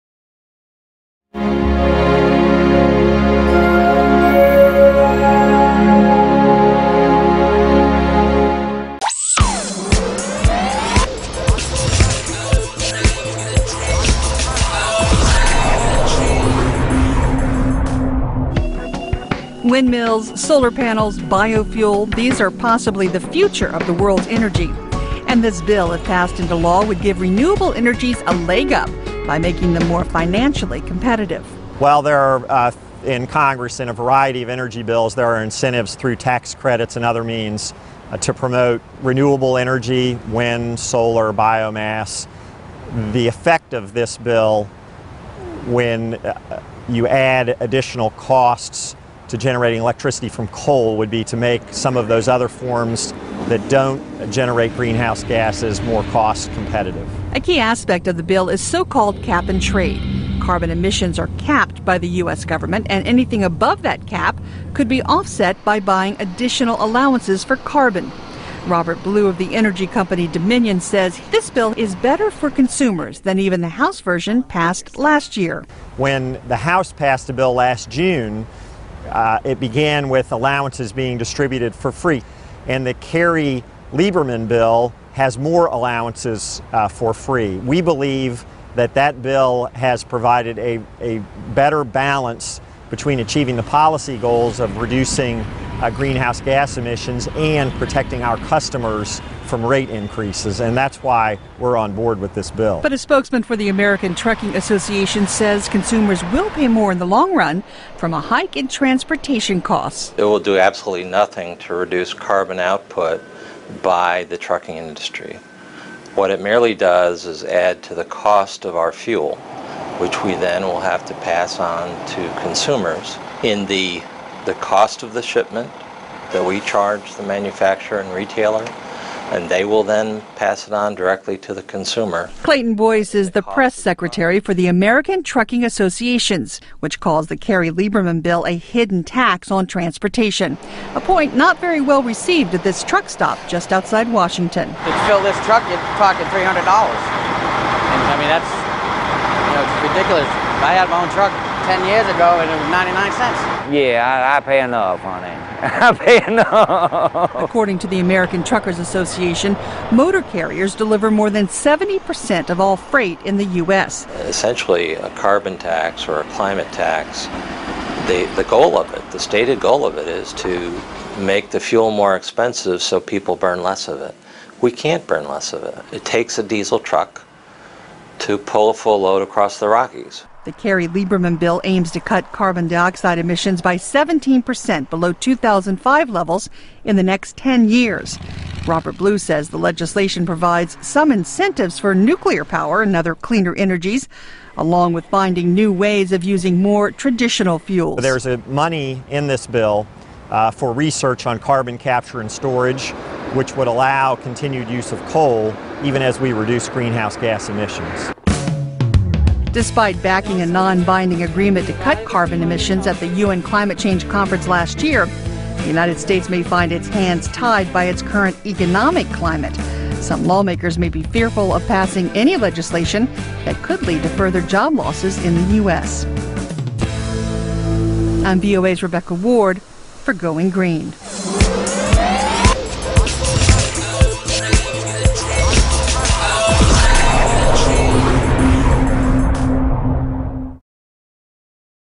Will Cap and Trade Cut US Carbon Emissions?
A new bill in the U.S Senate aims to cut carbon emissions by 17 percent in the next five years.  The United States agreed to a non-binding resolution to cut carbon emissions last year at the U-N Climate Change Conference.